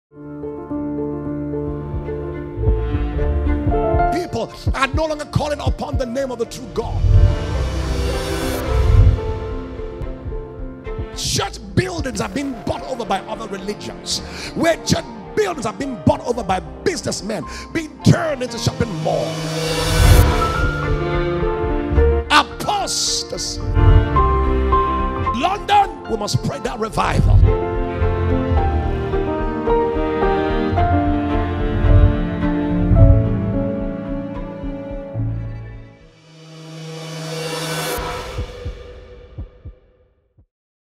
RIHA-CC 2019 LONDON
Hello London!
Join Apostle Goodheart Obi. Ekwueme at Revival Is Here Again City Conference (RIHA-CC) 2019 edition on Saturday 12th October.
Time: 9am
Venue: The Regents Conference and Events, Inner Circle, Regents Park, London NW1, 4NS.

ApostleGoodheart RIHA-CC#London#GPPA#HORM